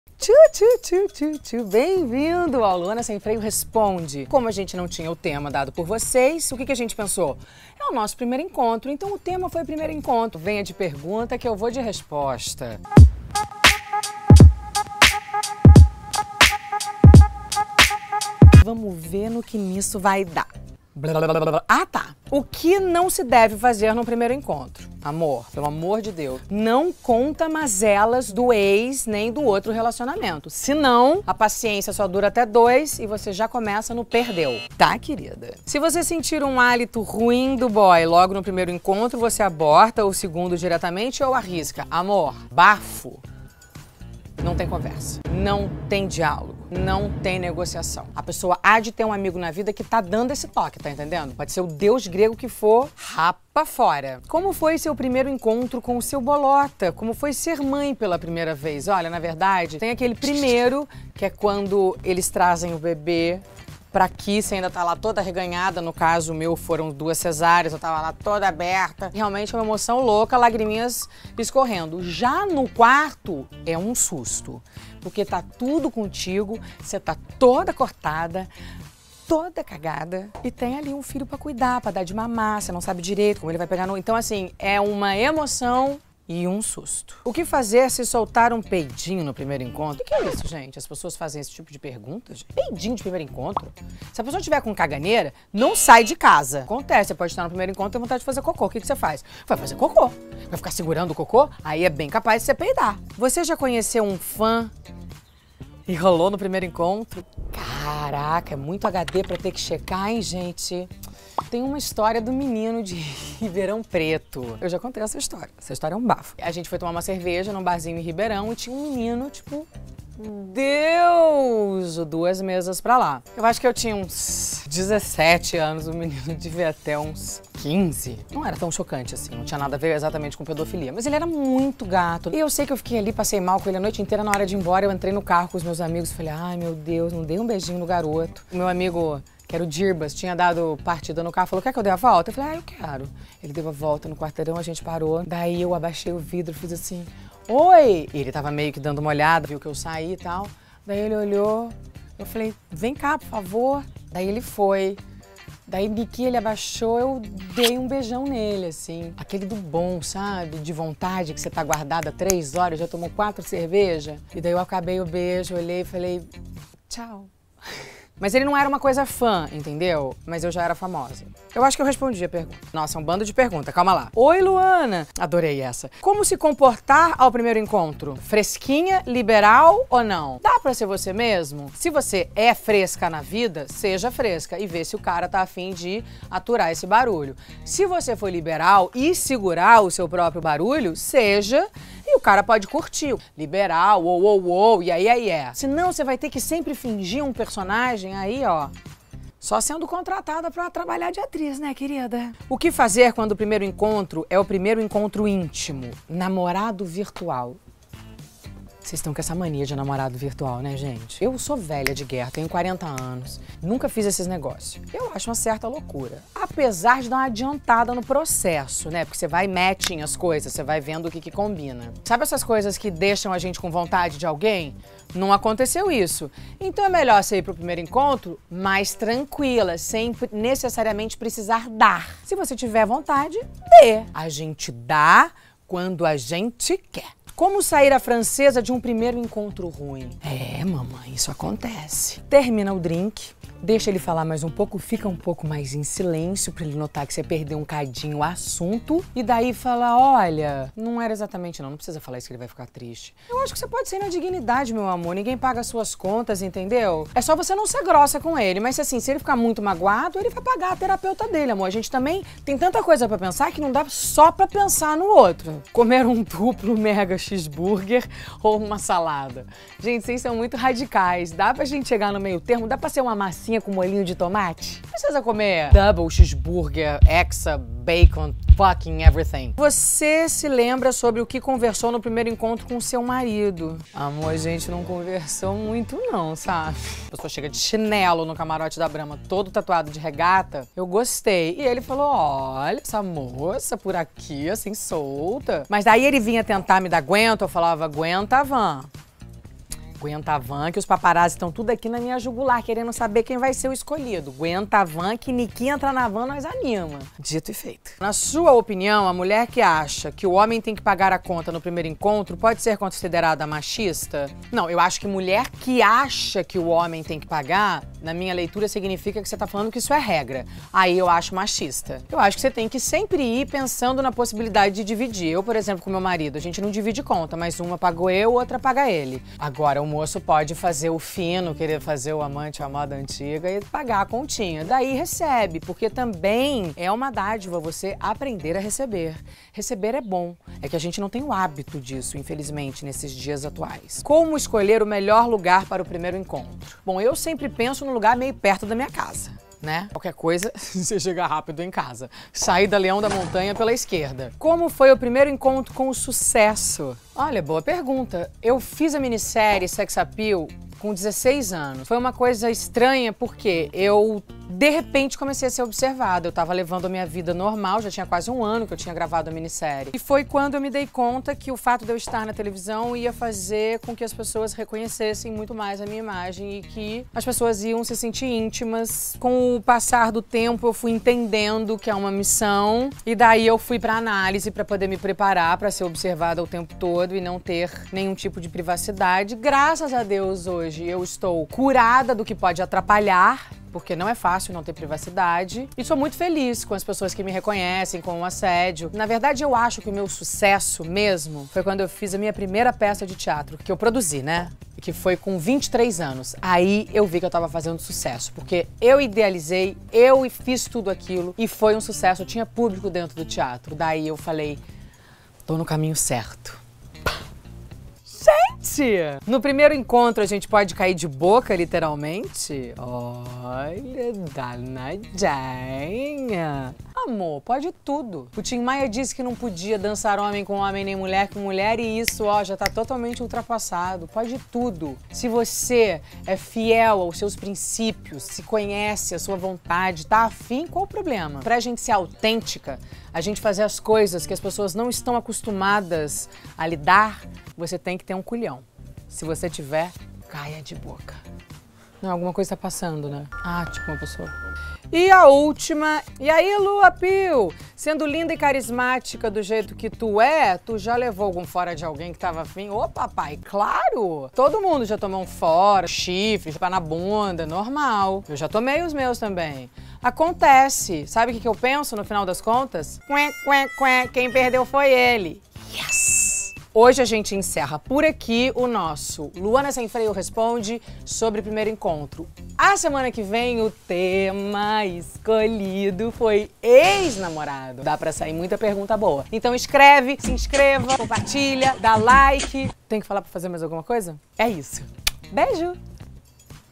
❤️‍ PRIMEIRO ENCONTRO - Luana Responde #1 (ESTREIA)
Se prepara, papai, que a Luana aqui vai responder pergunta da internet inteira! O tema de hoje é PRIMEIRO ENCONTRO. Solta o freio e vambora! Ah, e semana que vem, vamos falar de EX NAMORADOS, então já deixa a perguntinha aqui embaixo que eu vou ler tudo.